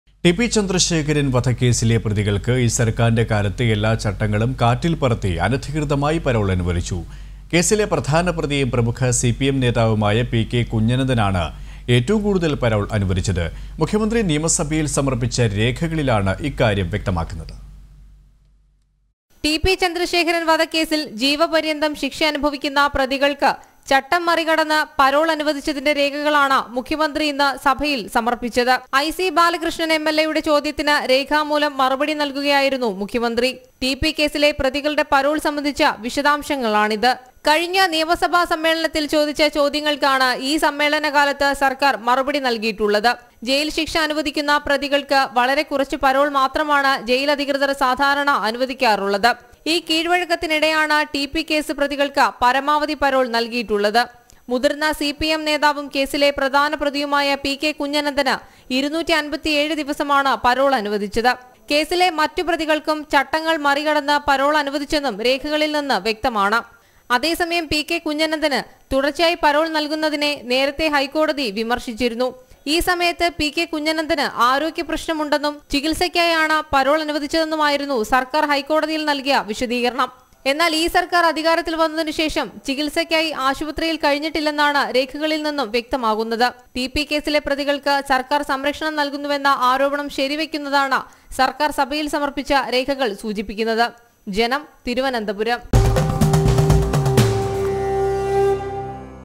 ചട്ടങ്ങൾക്ക് പുല്ലുവില : ടി പി കേസ് പ്രതികൾക്ക് വാരിക്കോരി പരോൾ
ടി പി ചന്ദ്രശേഖരൻ വധക്കേസിലെ പ്രതികൾക്ക് ഈ സർക്കാരിന്‍റെ കാലത്ത് എല്ലാ ചട്ടങ്ങളും കാറ്റിൽ പറത്തി അനധികൃതമായി പരോൾ അനുവദിച്ചു. കേസിലെ പ്രധാന പ്രതിയും പ്രമുഖ സിപിഎം നേതാവുമായ പികെ കുഞ്ഞനന്തനാണ് ഏറ്റവും കടുതൽ പരോൾ അനുവദിച്ചത്.